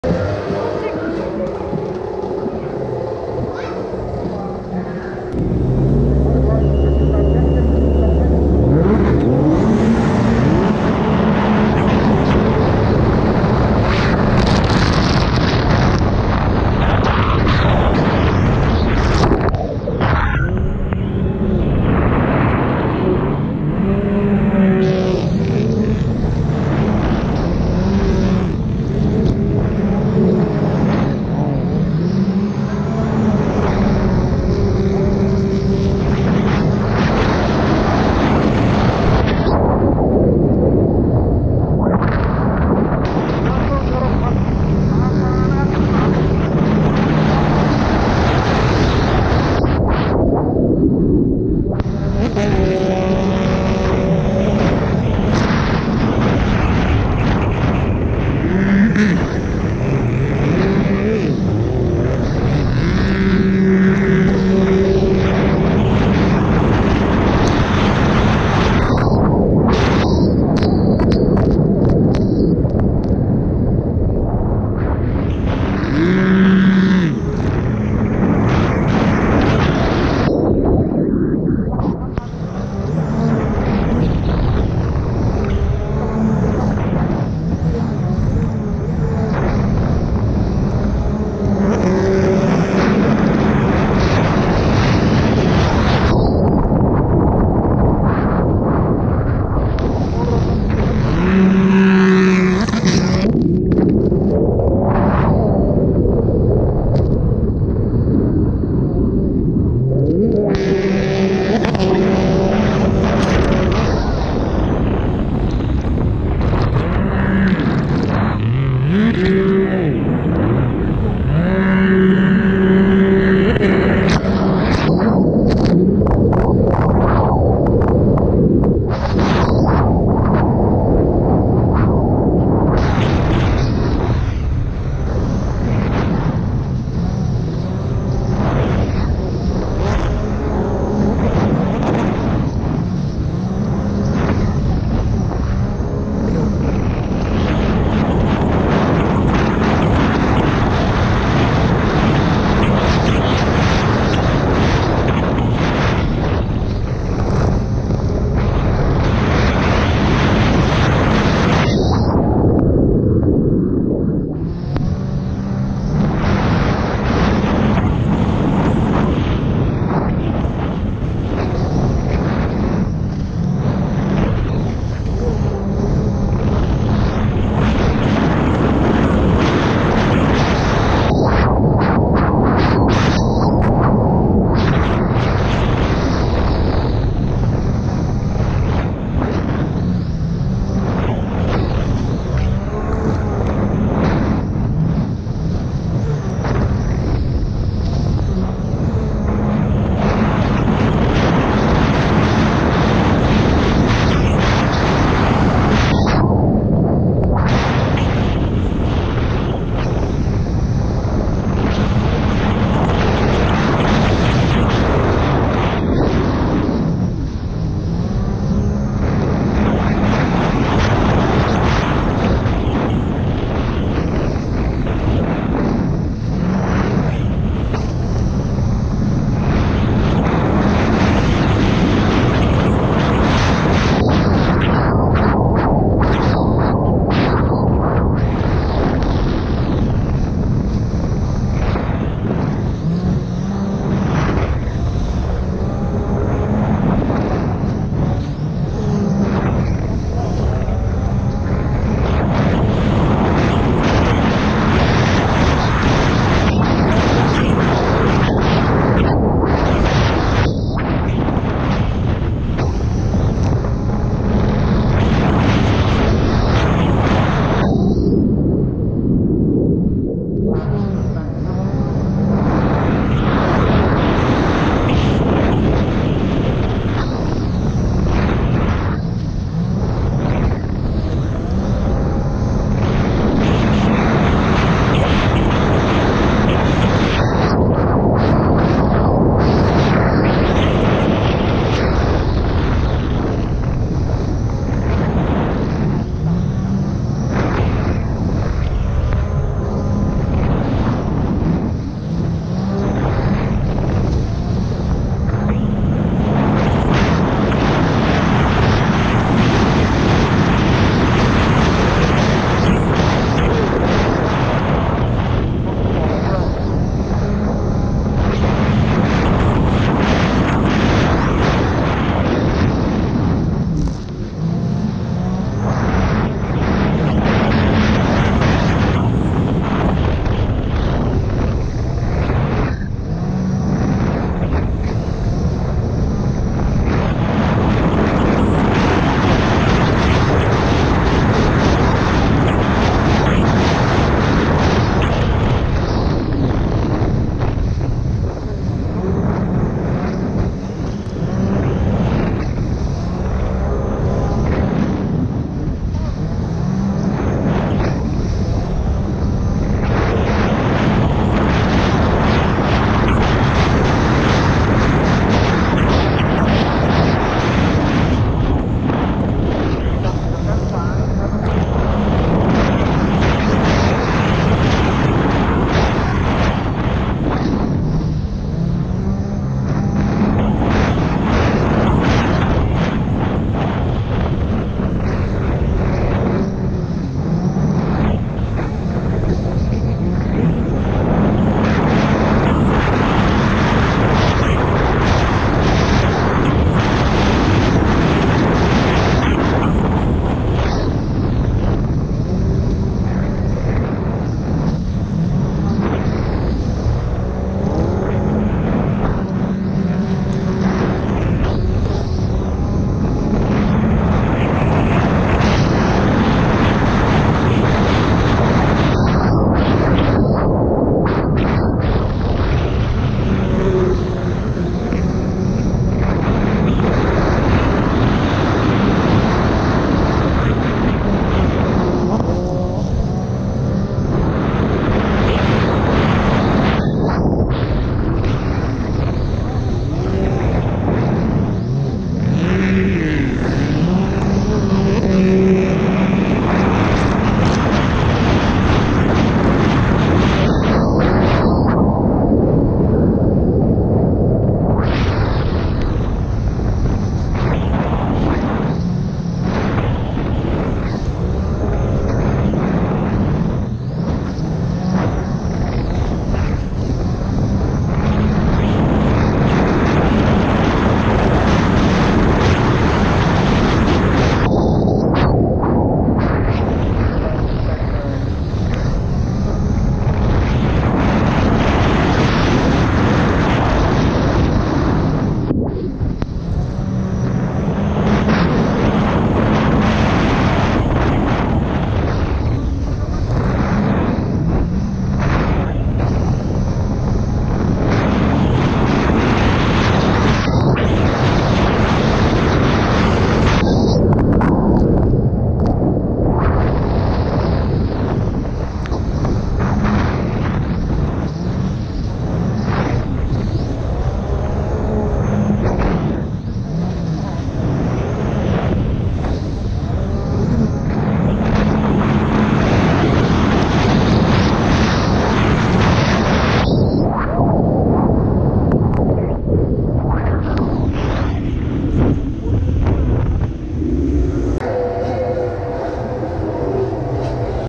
Pro Runabout GP, 2021 Chachiangsoa
Finishing 3rd! with the back up Stock Runabout! 2021 RXPX